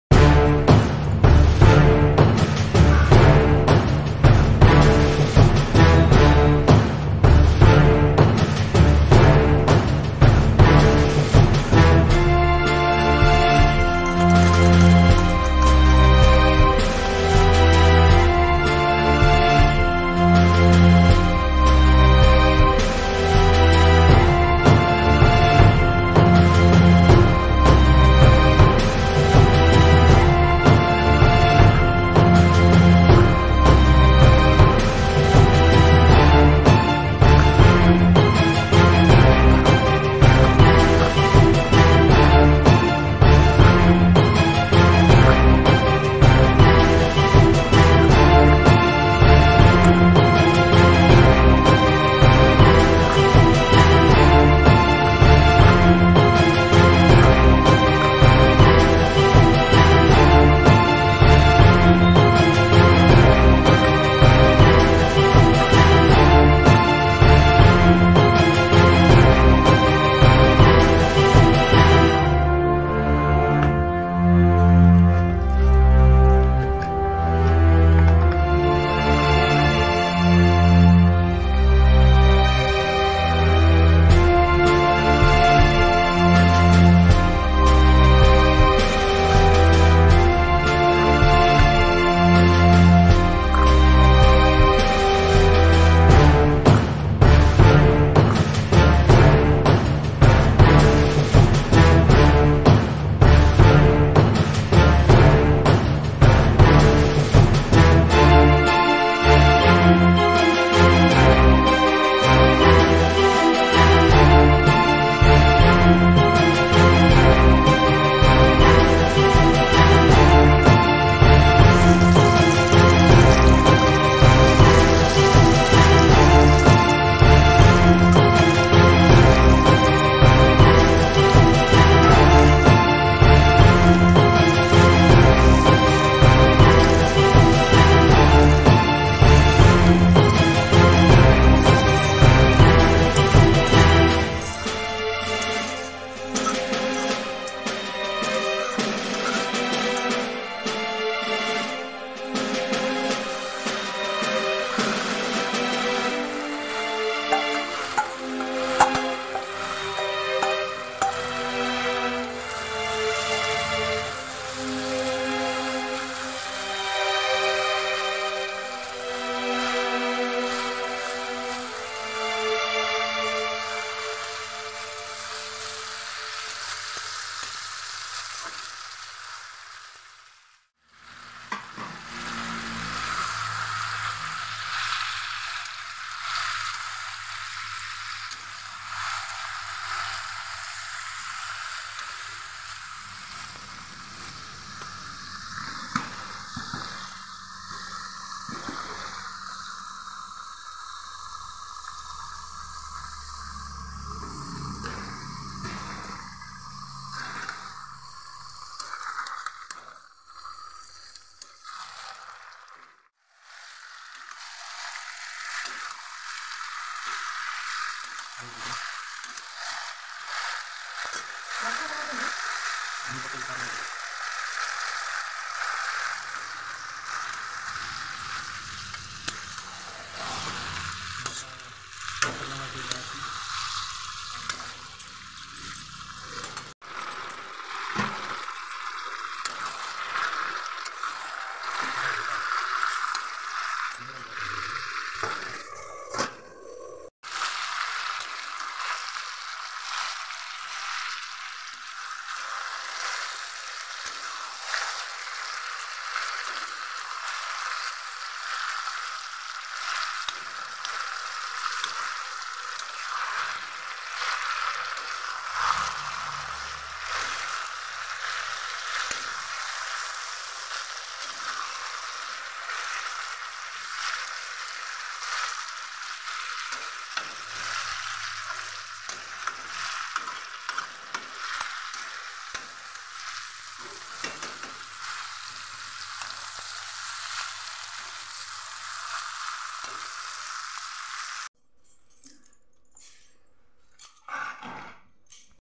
5 Best Chicken Curry Recipes I chicken curry I eating show I
This video based on Chicken 🐓 curry recipe and ideas of making best chicken curry recipe whith short period of time. Watch and enjoy the the video.
5 Best Chicken Curry Recipes I chicken curry I eating show I
                              thanks